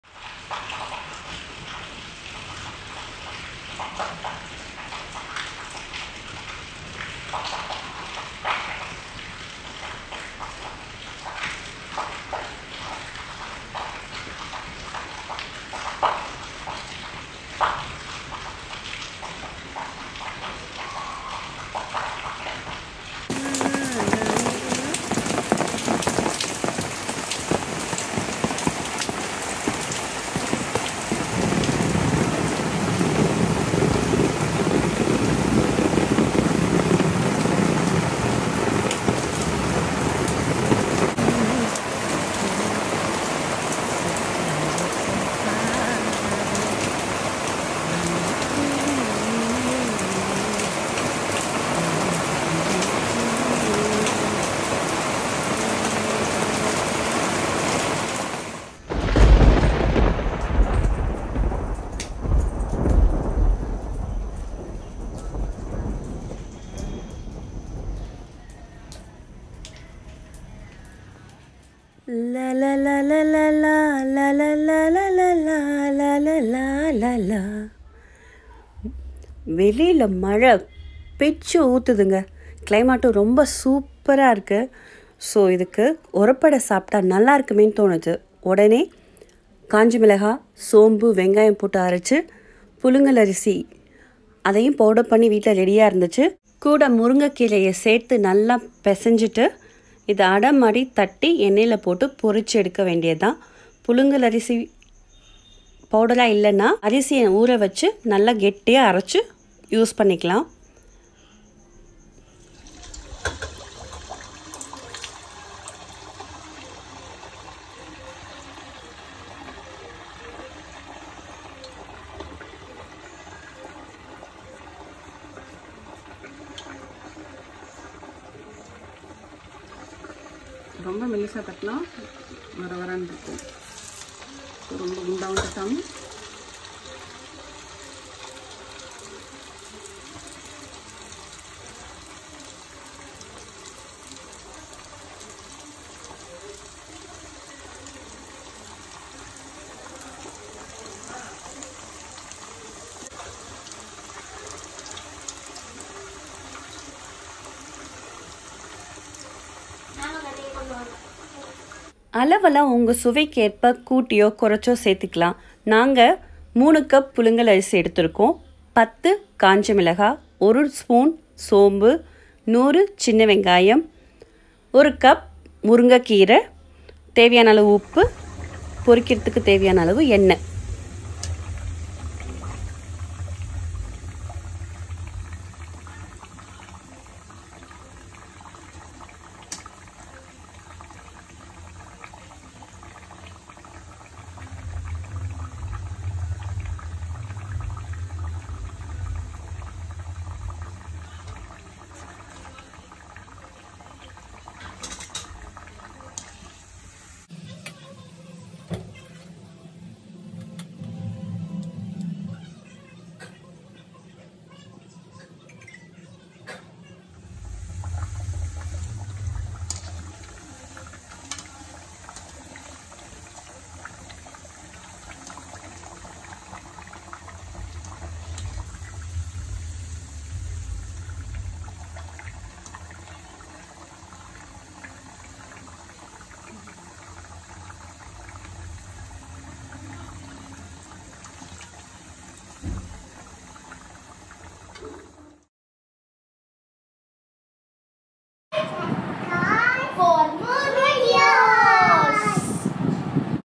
ORAPPU ADAI | Easy breakfast & evening snacks | Traditional snacks for rainy season
Traditional Breakfast and snacks it’s every easy to prepare and tasty.

Ingredients:

Rice flour (or) soaked rice and grind to thick paste than use - 3 cups

Red chillies- 10 nos

Small onions - 100gms

Drumstick leaves - 1 cup

Salt according to our taste

Mix all the above ingredients thoroughly and make it to a thick paste

Oil as required to fry Adais

Do try this traditional ORAPPU ADAI